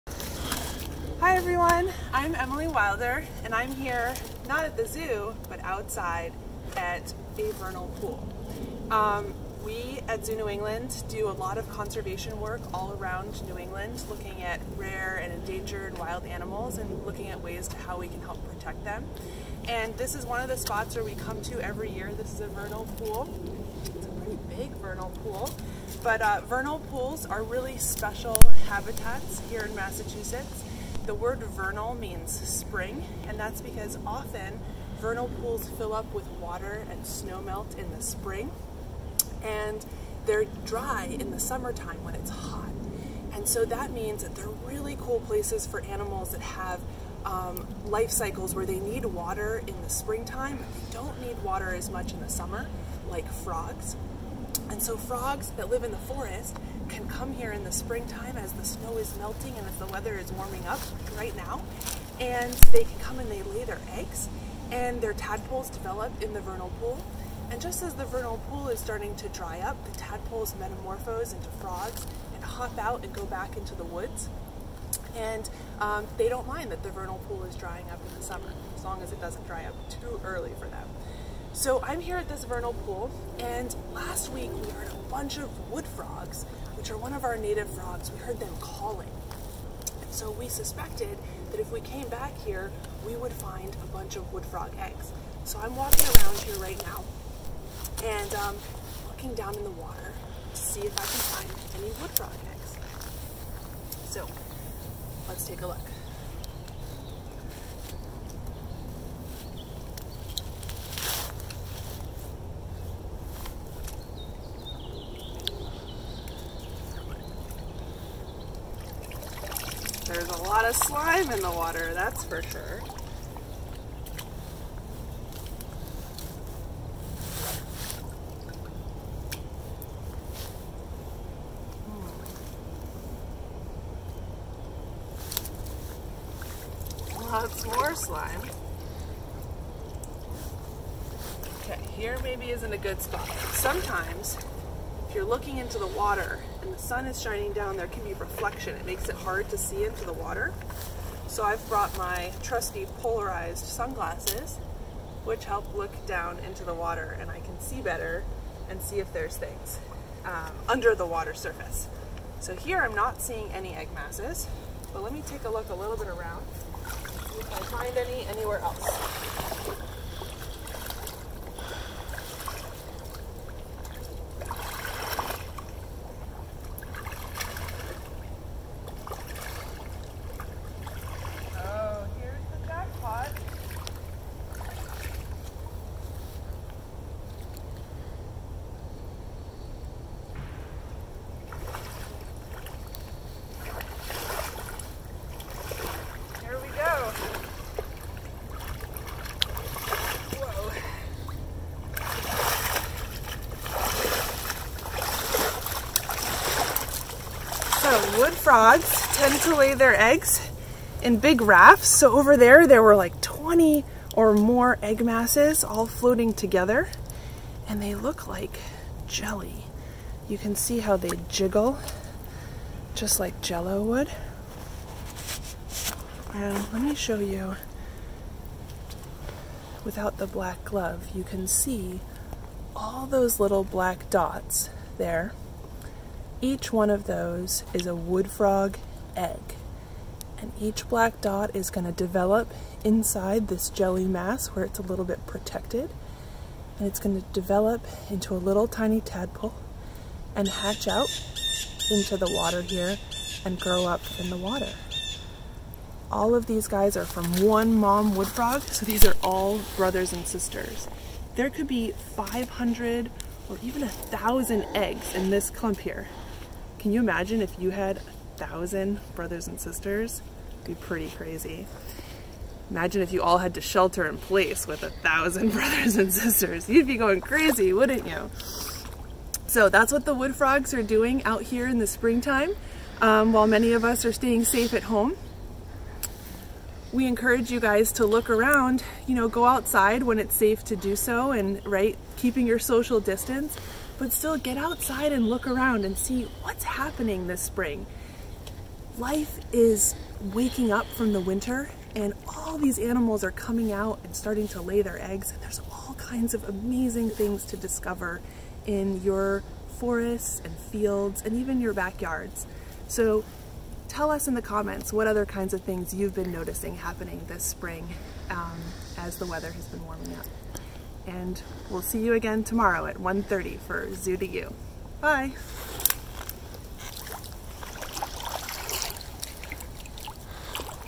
Zoo to You: Visit a vernal pool
Today’s ZooToYou comes from the vernal pool! Even during these tough times, members of our staff have been continuing to complete important field work. Emilie from Zoo New England's Grassroots Wildlife Conservation is here to talk about what the start of spring time means for native species that we’re helping to protect, like wood frogs, and will even give you an up-close look at wood frog eggs! Learn more about Grassroots Wildlife Conservation's amphibian conservation work